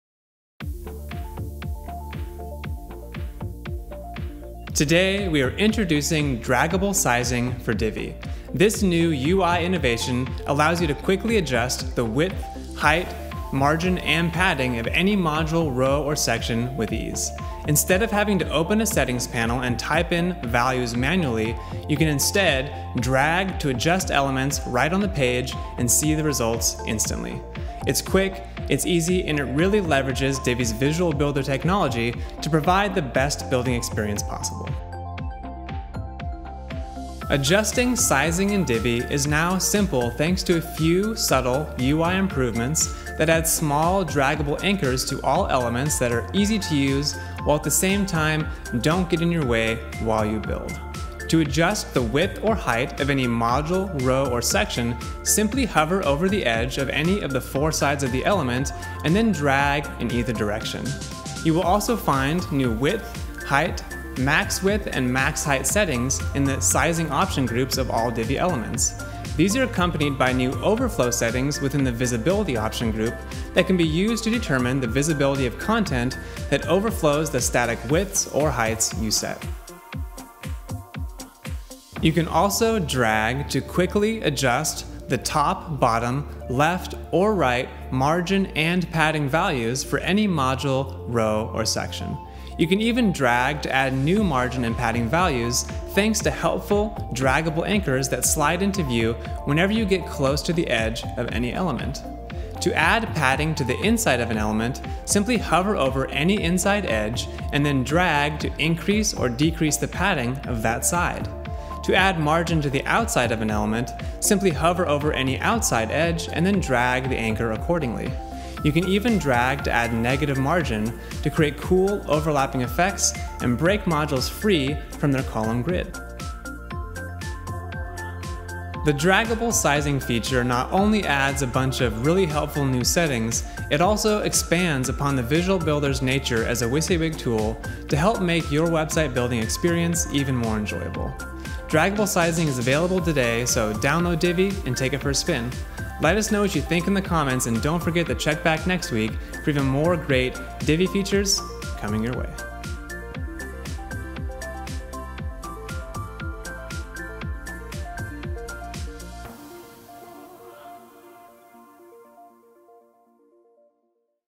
Introducing The New Draggable Sizing And Spacing Controls For Divi
Today we are introducing Draggable Sizing for Divi. This new UI innovation allows you to quickly adjust the width, height, margin and padding of any module, row or section with ease. Instead of having to open a settings panel and type in sizing values manually, you can instead drag to adjust the elements right on the page and see the results instantly. It’s quick, it’s easy and it really leverages Divi’s Visual Builder technology to provide to best building experience possible.